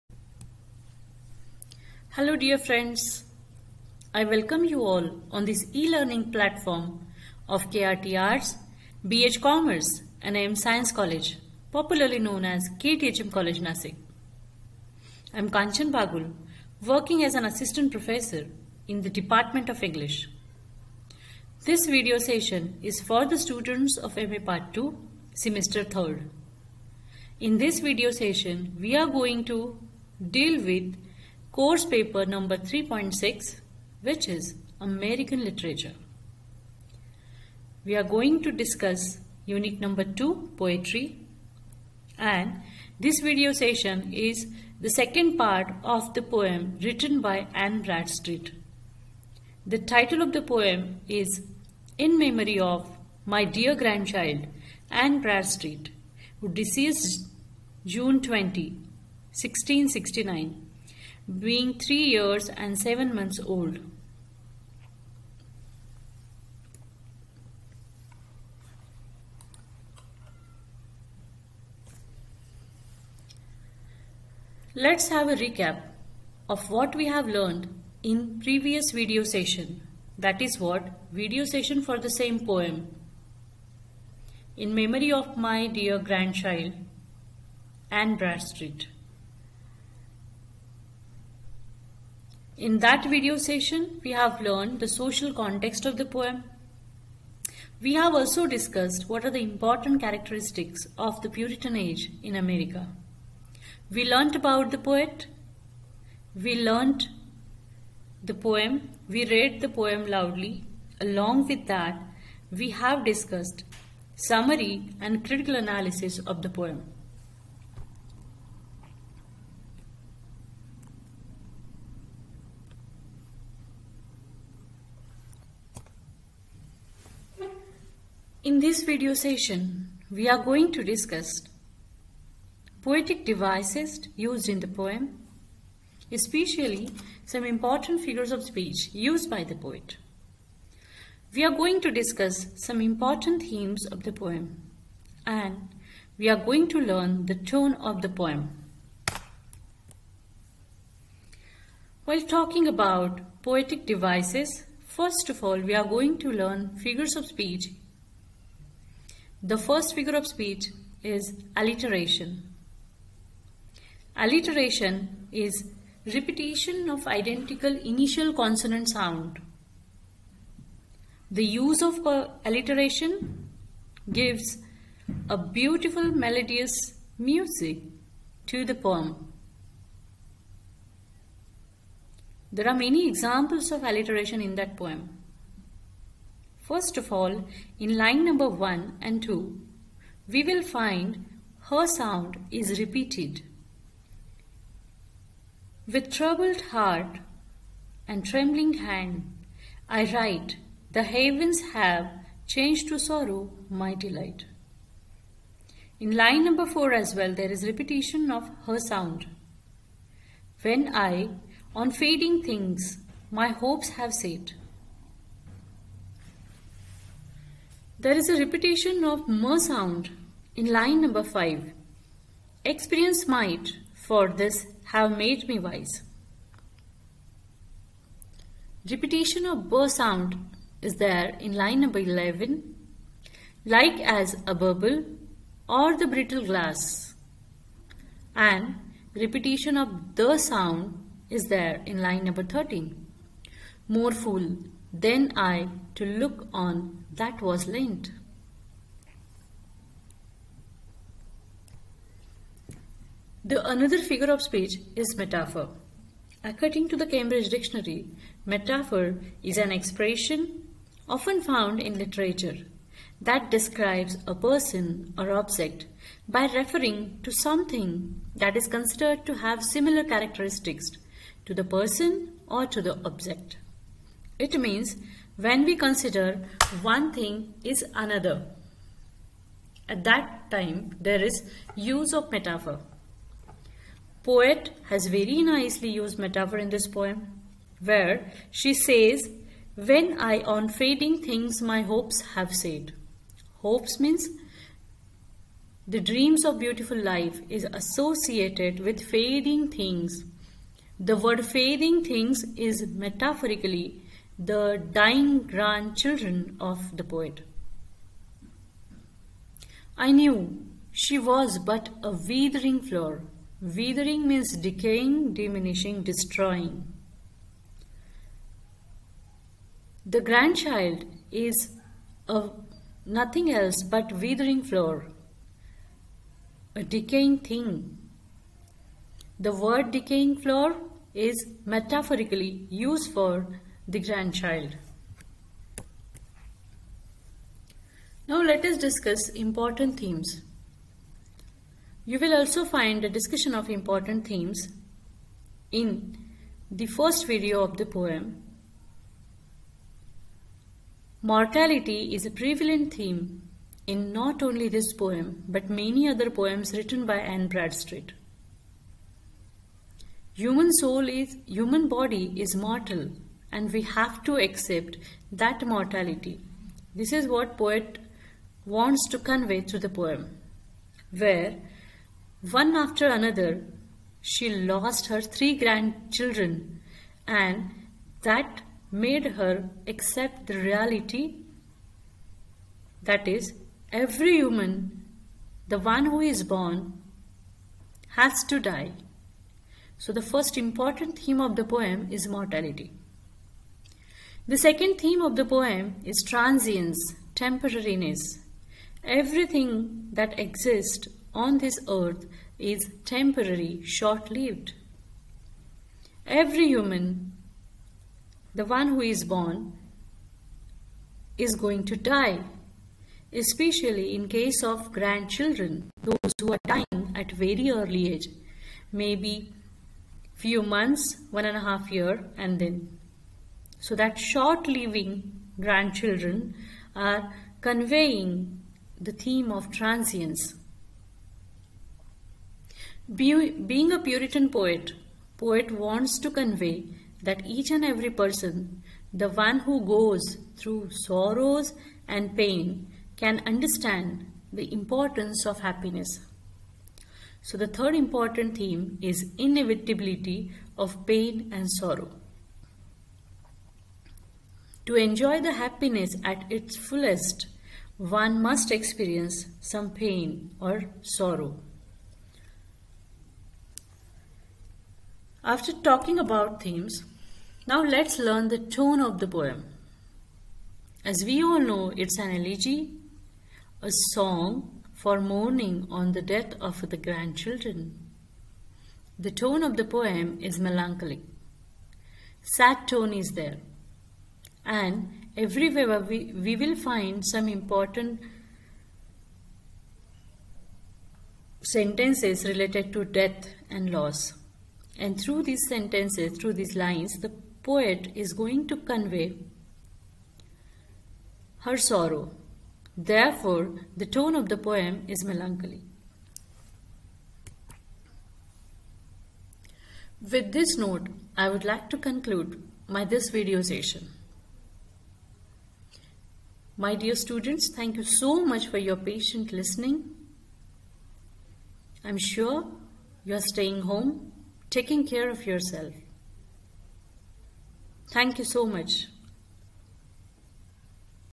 M A II | Sem III | 3.6 American Literature | Ann Bradstreet| Kanchan Bagul
Unit : 02 Poetry
Topic: In Memory of My Dear Grandchild Anne Bradstreet, Who Deceased June 20, 1669, Being Three Years and Seven Months Old